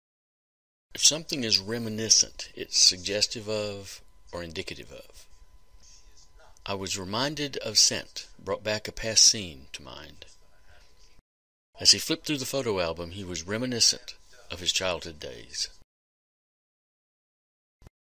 reminiscent
This video will define reminiscent, use it in a sentence, and show a mnemonic device which makes easier to remember.